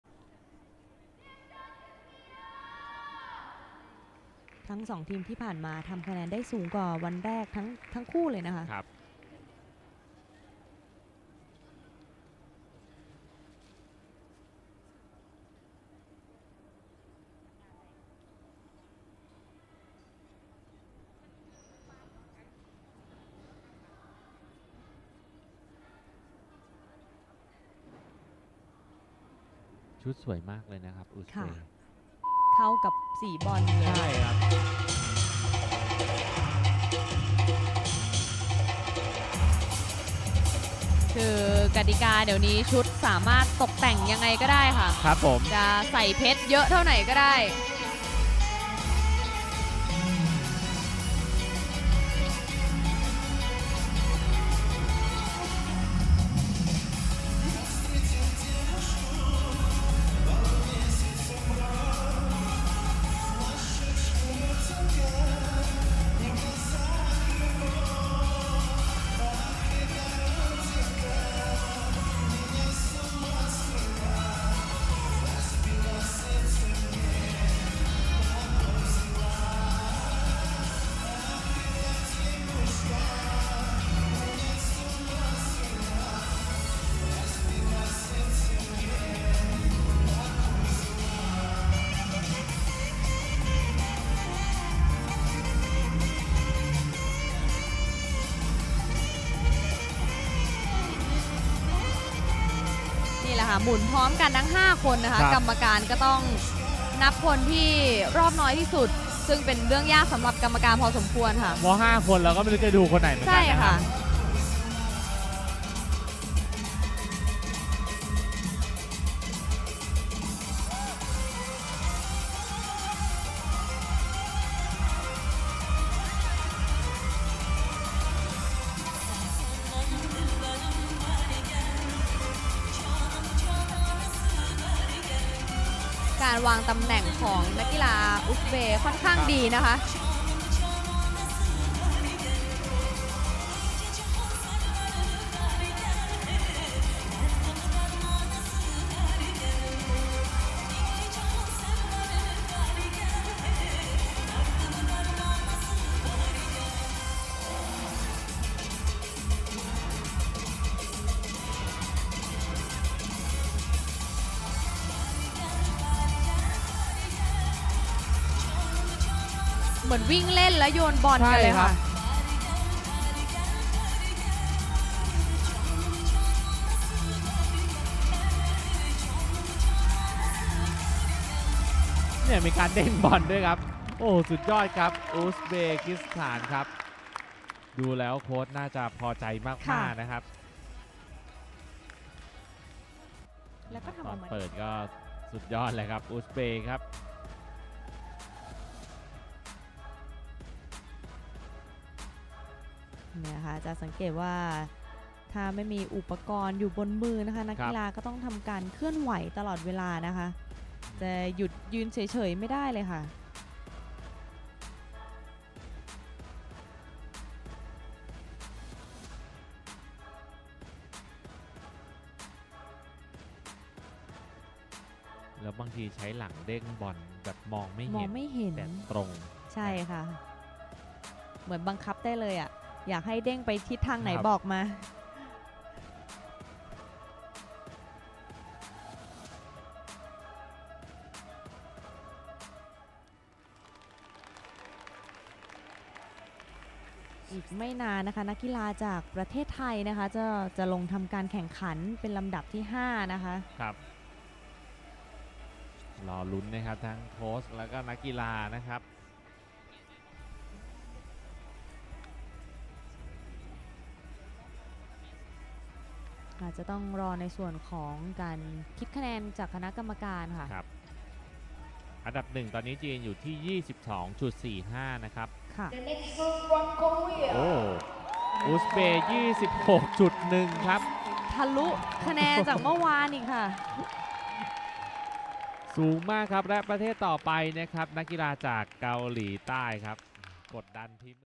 22 06 19 Сборная Узбекистана Финал Мячи Золото
Чемпионат Азии по художественной гимнастике. Групповые упражнения